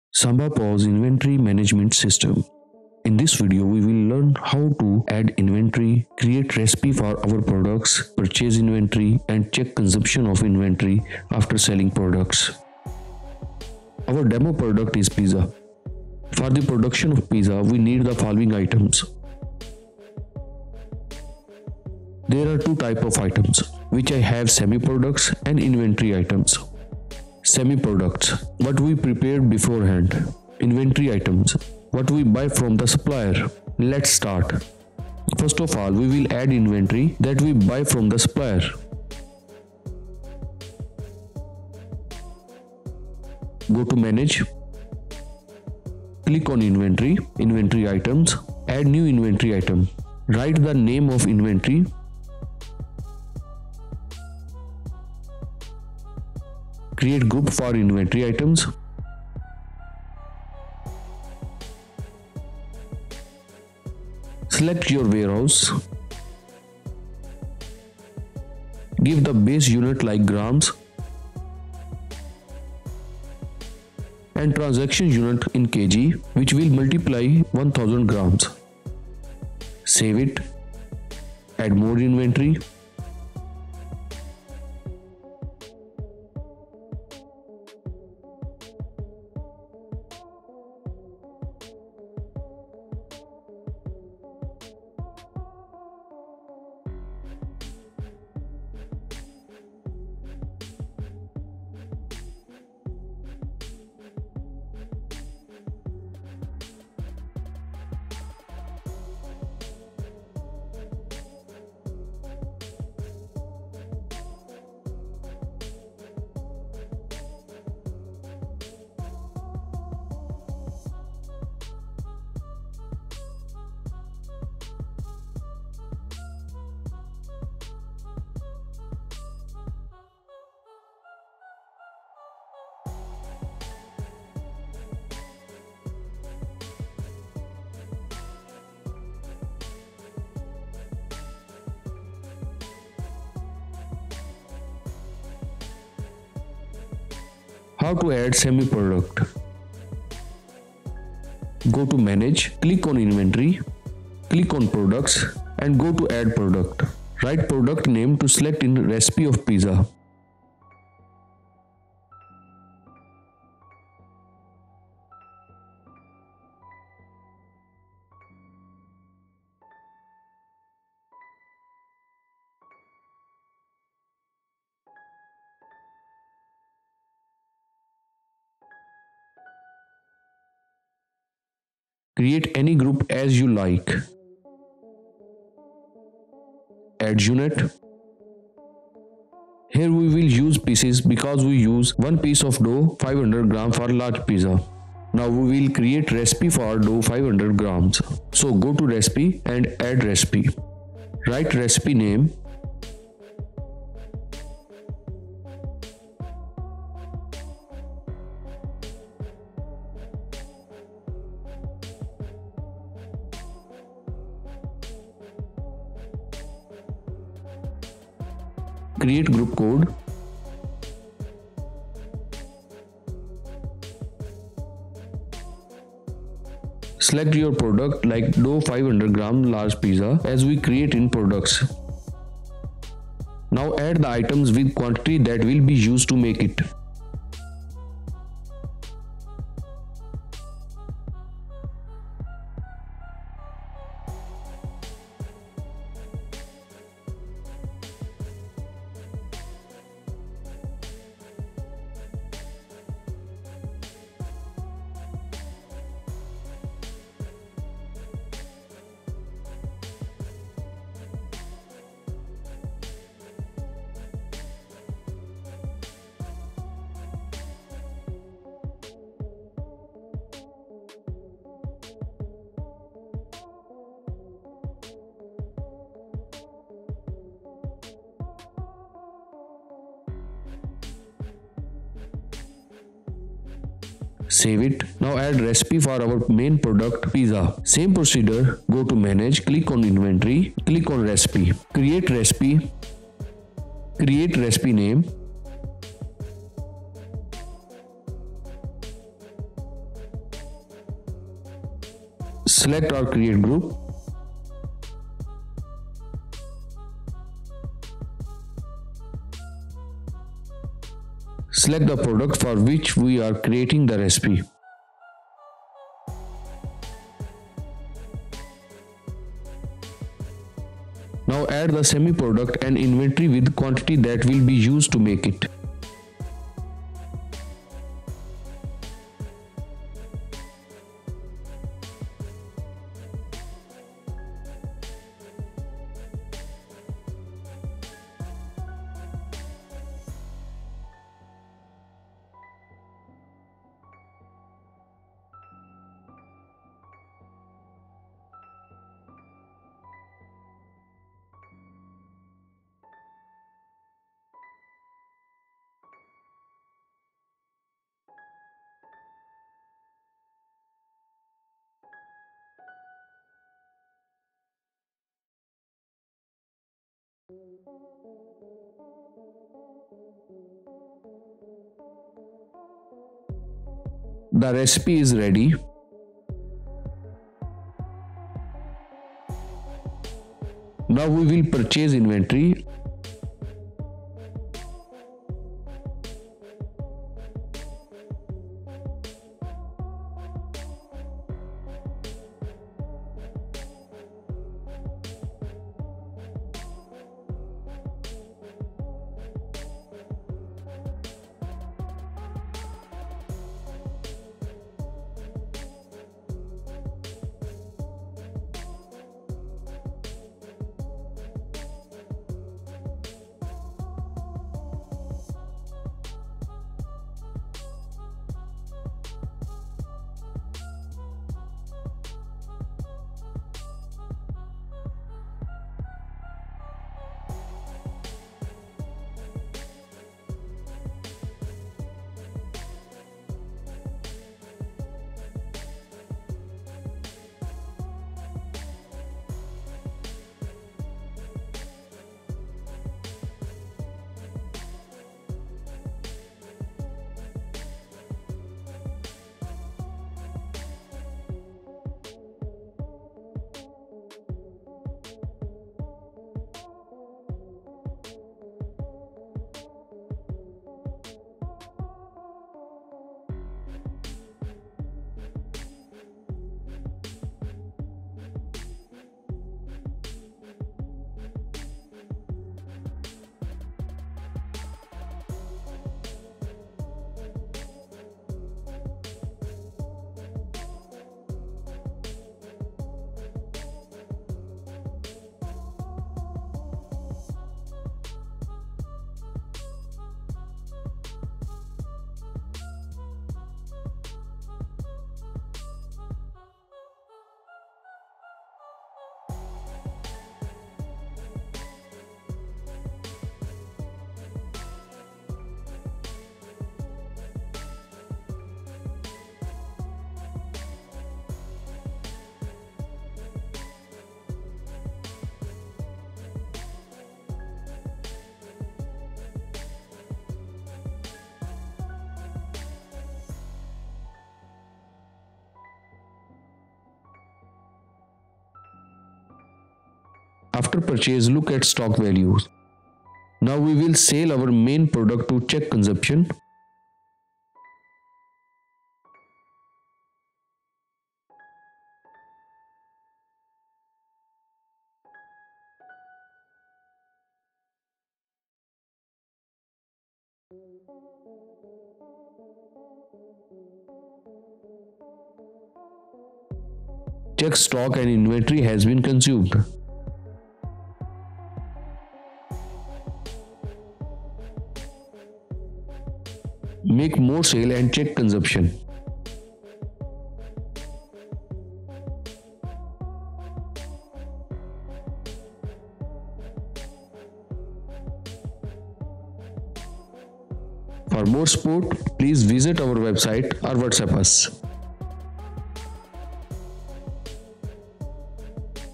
How to Inventory Management in Samba POS
Samba POS Restaurant and Inventory Management System
You will learn how to manage you inventory in Samba POS.